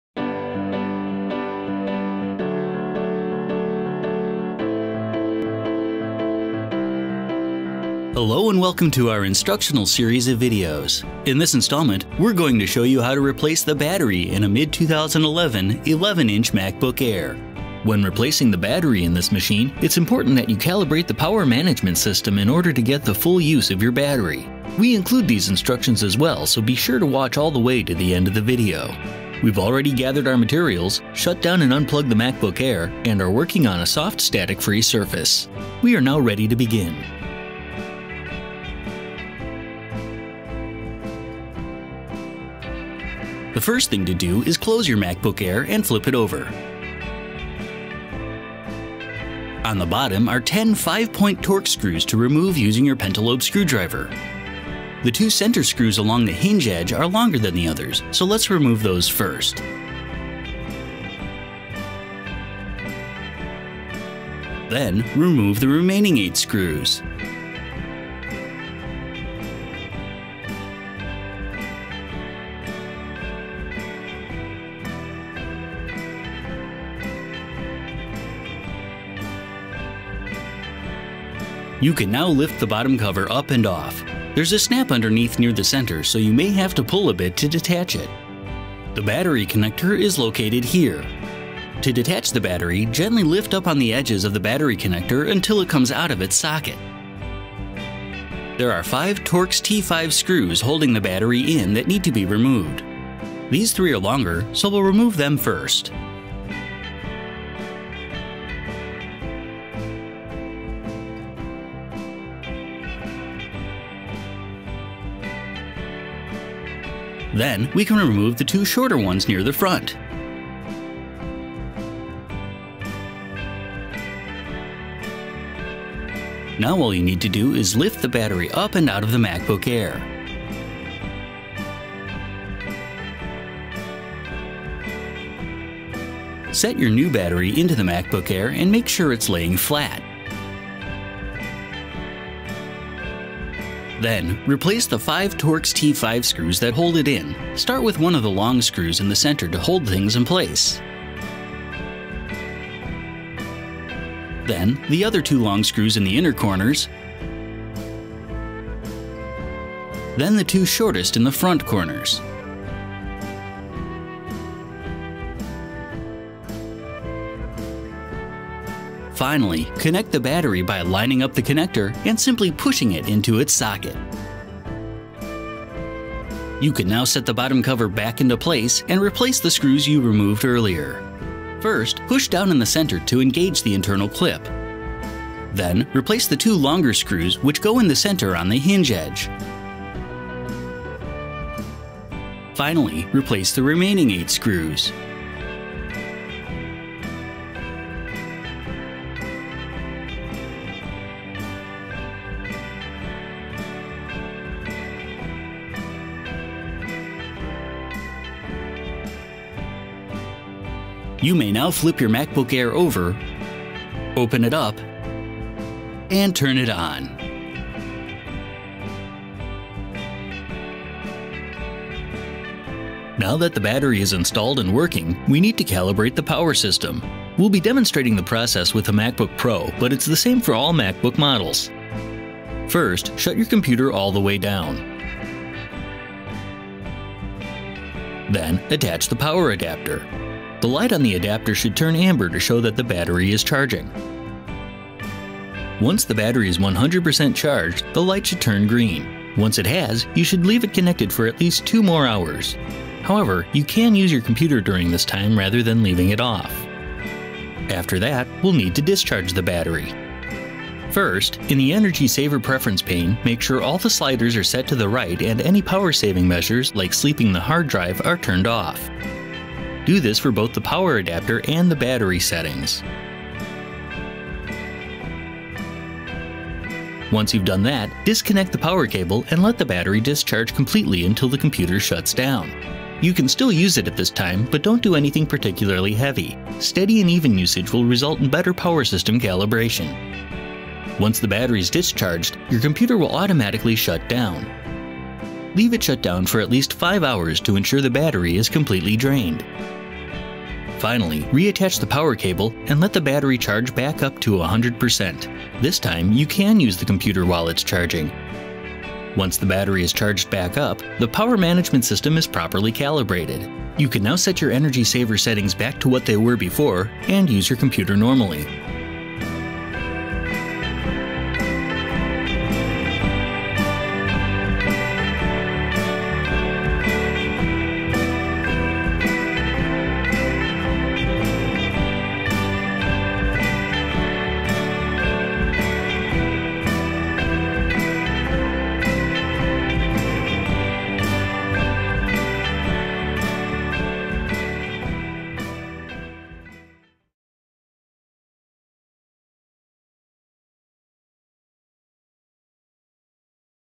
MacBook Air 11" 2011 Batterij vervangen
In deze video laten wij zien hoe u de batterij van de Macbook Air 11" 2011 vervangt.

Heeft u geen zin om zelf met een schroevendraaier aan de slag te gaan?